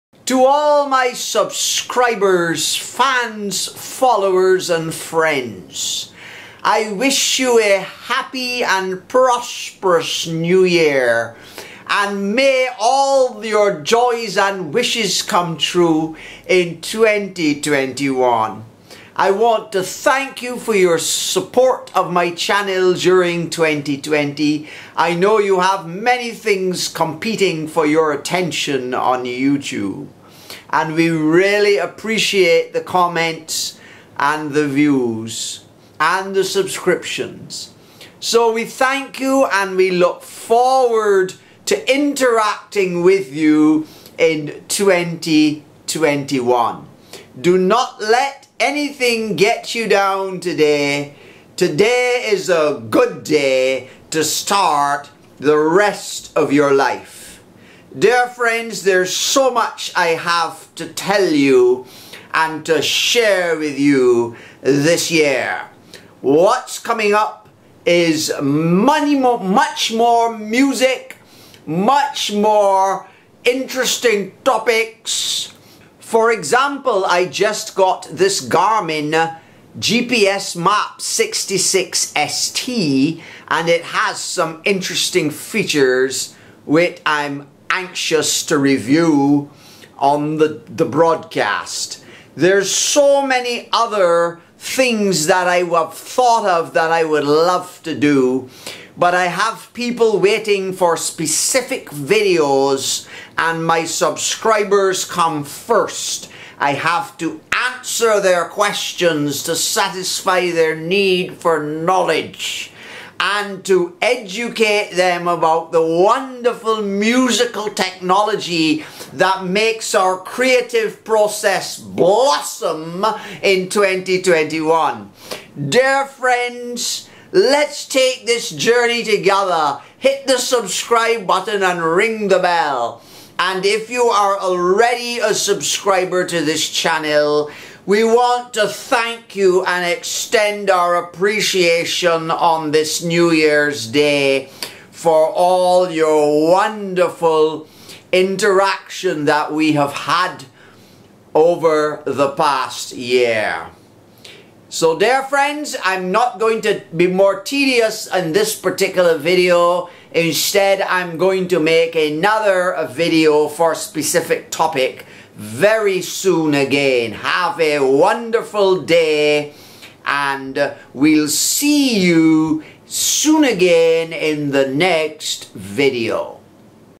New Year's Message to all my Dear Friends on Social Media
Wishing all my viewers a Wonderful New Year 2021 and promising plenty videos to Educate, Enlighten and Entertain you in the coming year.